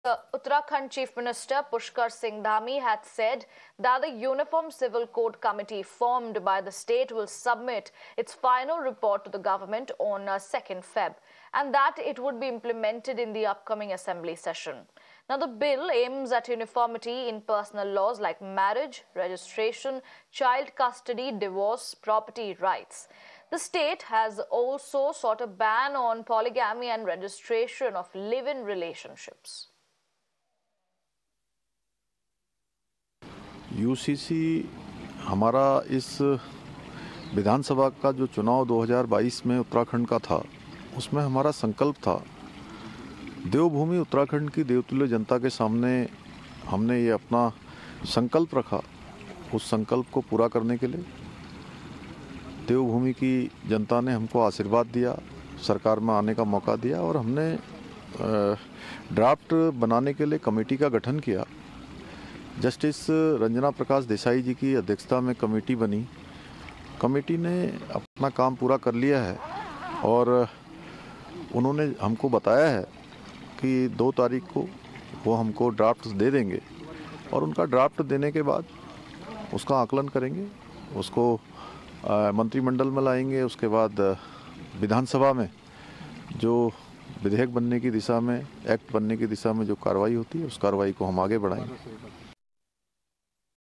'UCC In Uttarakhand Soon, To Be Tabled This Session', Promises CM Pushkar Singh Dhami | Top News
A five-member committee tasked with formulating a draft for the implementation of the Uniform Civil Code (UCC) in Uttarakhand is set to present its report to the state government on February 2, according to Chief Minister Pushkar Singh Dhami. He announced that the report will be placed before the state assembly during the upcoming session scheduled for February 5-8. The Uniform Civil Code aims to establish a unified legal framework encompassing diverse customs and traditions, governing matters such as marriage, divorce, inheritance, and maintenance.